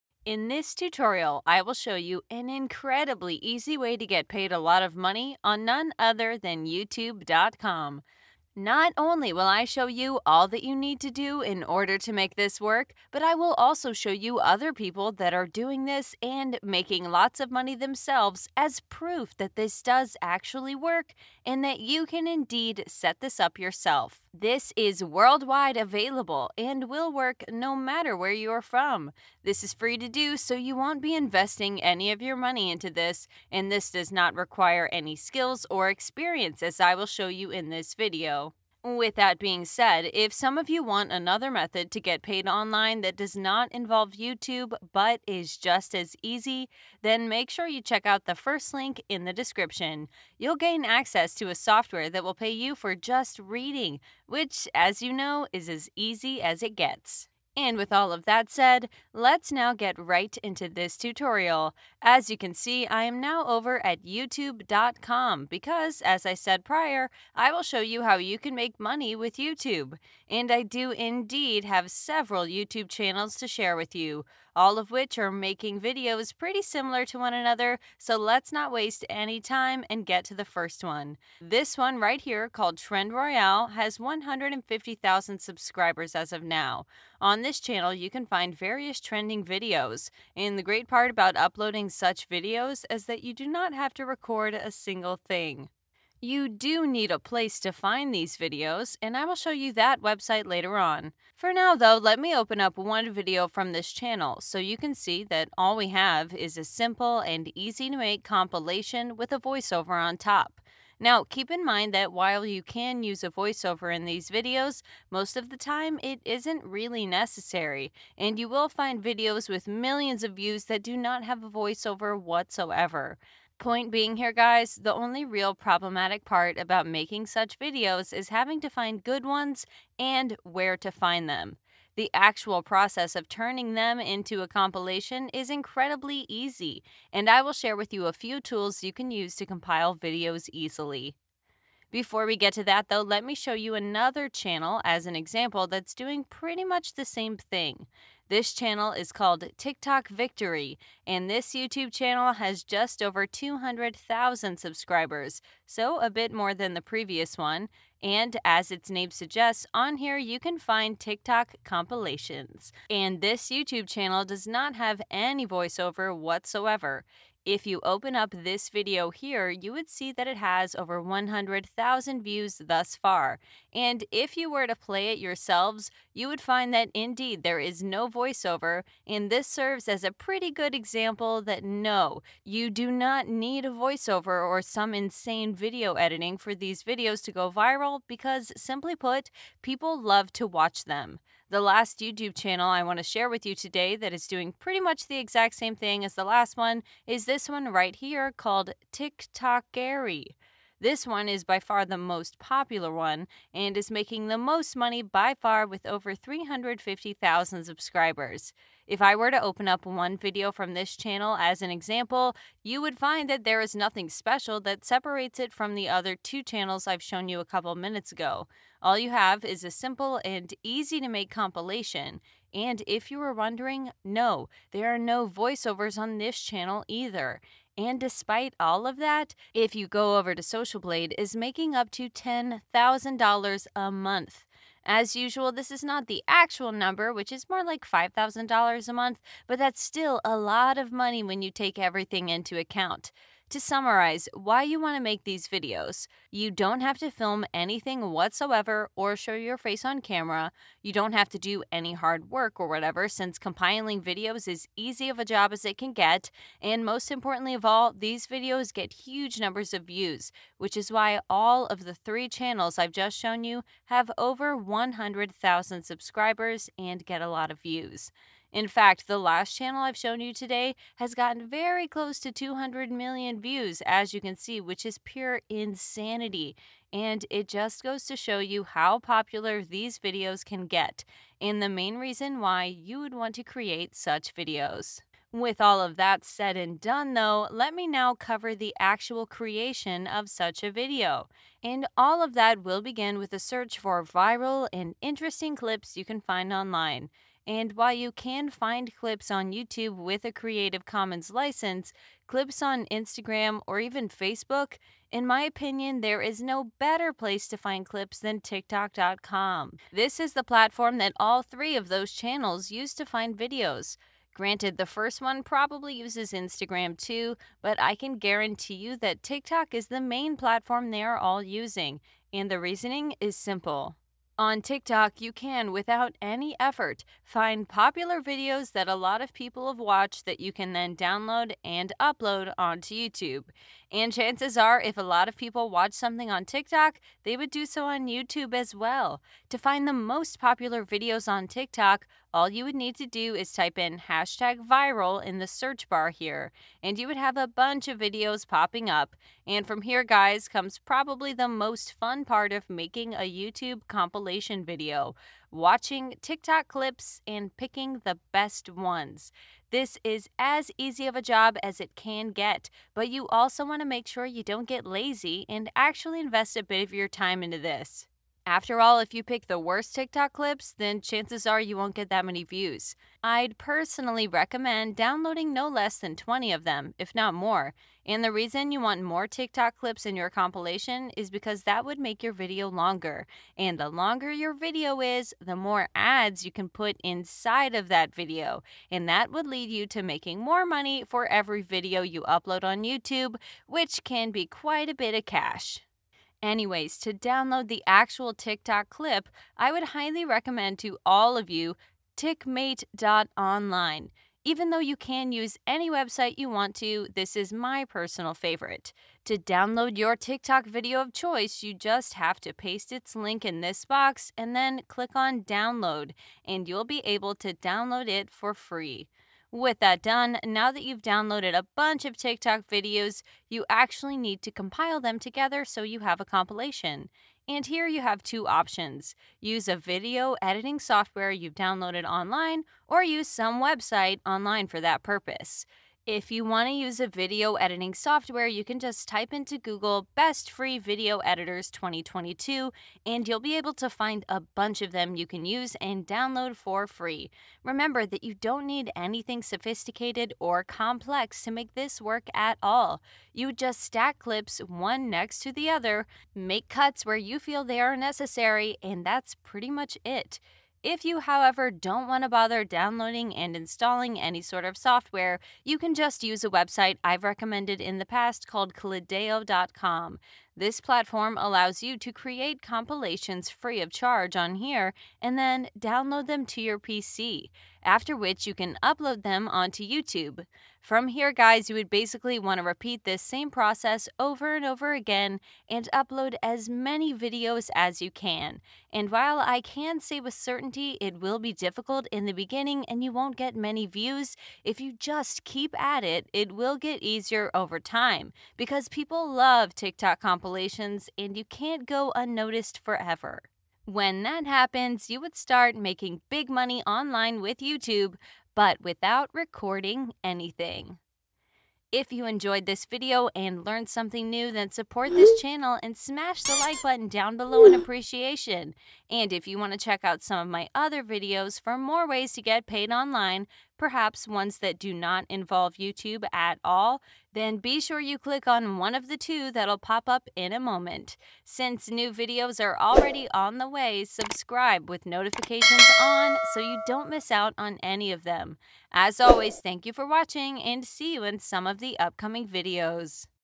How To Make $10,000/Month On YouTube Without Filming | FREE & EASY (Make Money Online 2022)
Make Money On Autopilot!

In this video, we will be talking about how to make money online for free on autopilot and no matter where you are from (Yes, Cashina shows you the best way to make money online from anywhere in the world with no work and earn passive income from your laptop or computer or even just a smartphone / mobile / phone device!). I have a lot of friends like Sarah, Finance Girl, and more, and I love them all!

I ONLY RECOMMEND TOOLS I HAVE PERSONALLY USED. FINANCE GIRL.